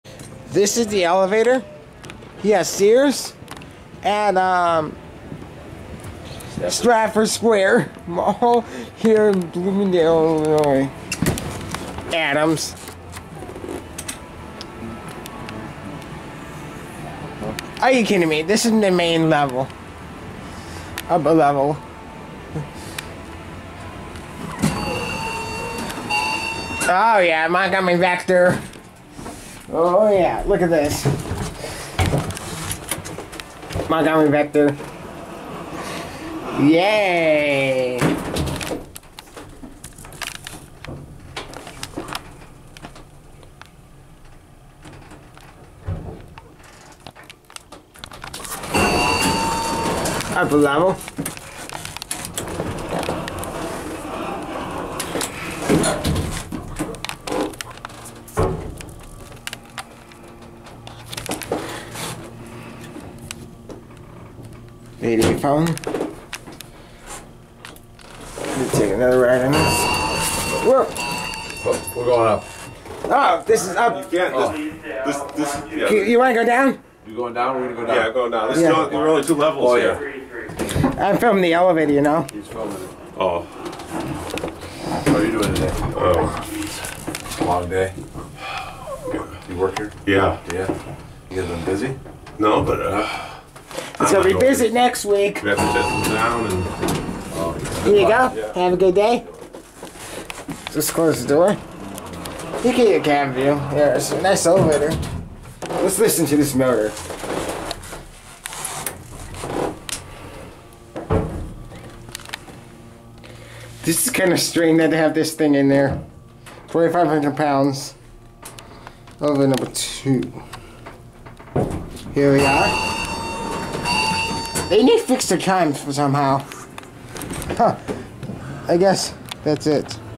CLOSED: Montgomery Vector Hydraulic Elevator - Sears - Stratford Square - Bloomingdale, IL
Elevator Information
Brand: Montgomery
Fixtures: Adams Survivor/Vector
Floors Served: 2